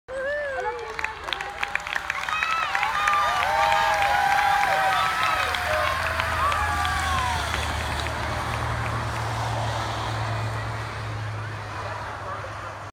Tour de France 2019 mountain stage (continued)
A pack of riders descends from stage 13 through the Pyrenees mountains​ in France.